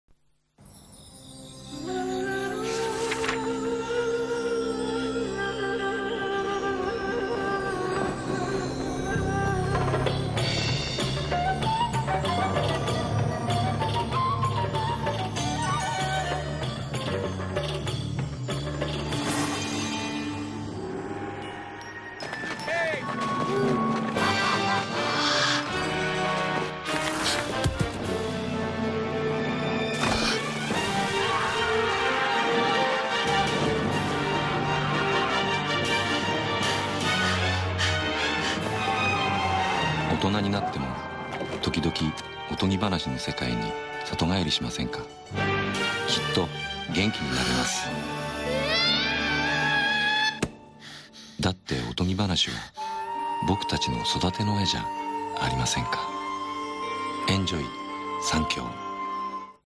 SANKYO Commercial "Arabian Night" 1'00"(June Chikuma plays Nay)
Japanese TV commecrial video by SANKYO /
June Chikuma plays Nay. /
SANKYO CM アラビアンナイト篇 /
竹間ジュン(ナイ)